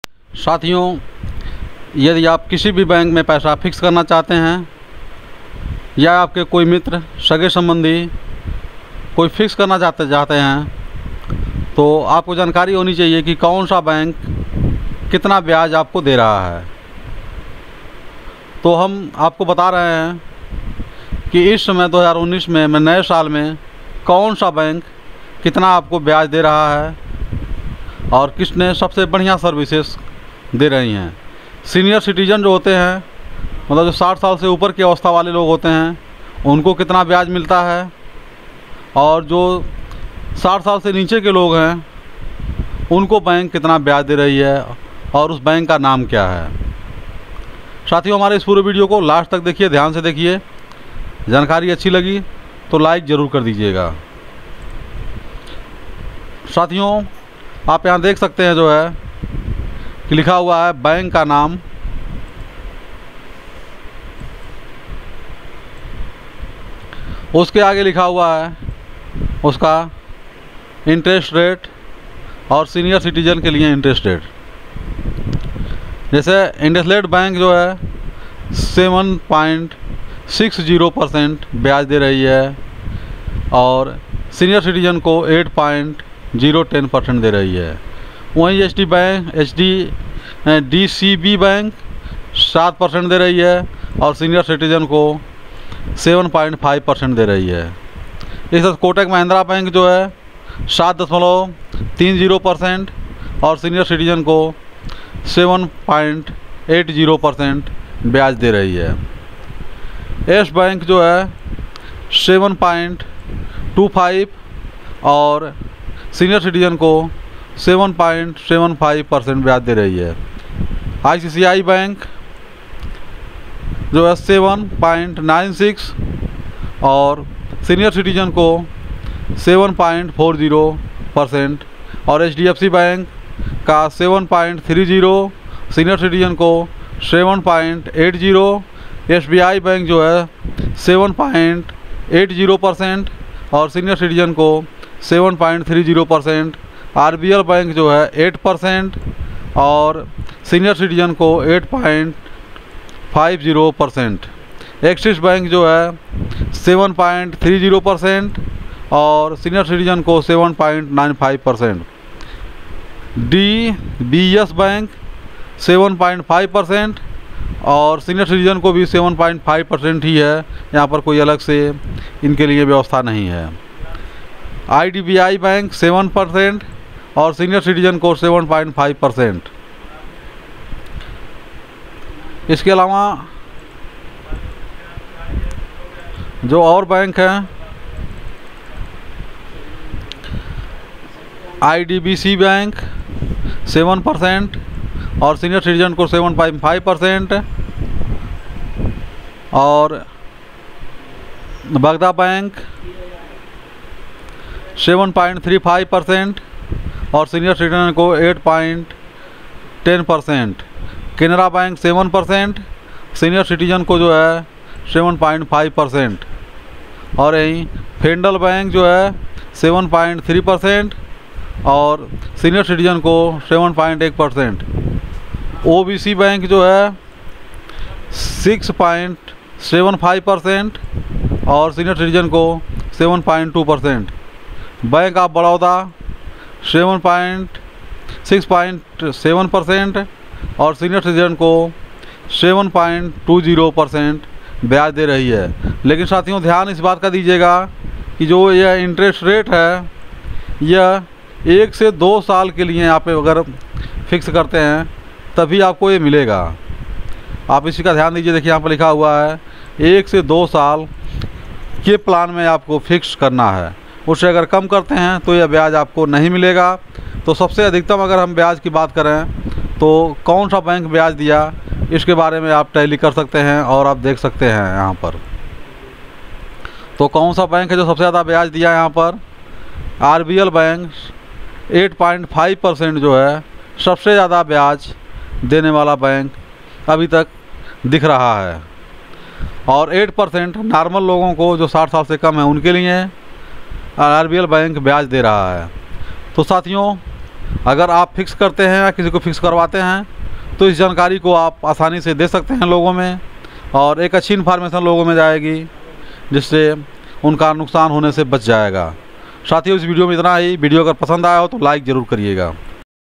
किस बैंक में FD पर सबसे ज्यादा ब्याज मिलता, kon sa bank sabse jyada byaj deta hai
बैंक बाजार की टीम ने विदेशी बैंकों से लेकर देश के सरकारी और प्राइवेट बैंकों की एफडी पर ब्याज दरों का ब्योरा जुटाया है। इसमें 1 करोड़ रुपये से कम की राशि पर कौन सा बैंक किस दर से ब्याज देता है, इसका बारीकी से ब्योरा दिया गया